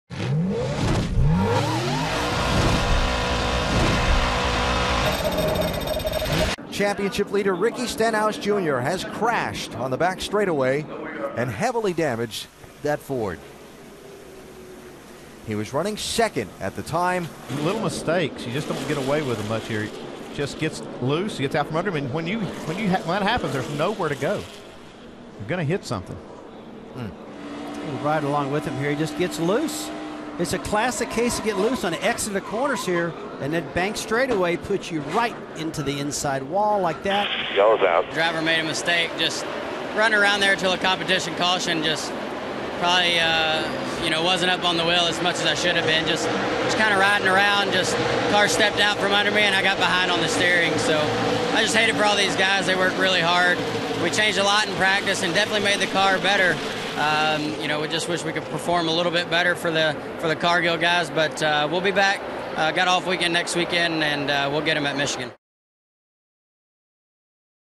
Driver mistake costs Ricky Stenhouse
Ricky Stenhouse Jr. states he was riding around until the competition caution, when the car 'stepped out,' sending the No. 6 into the wall.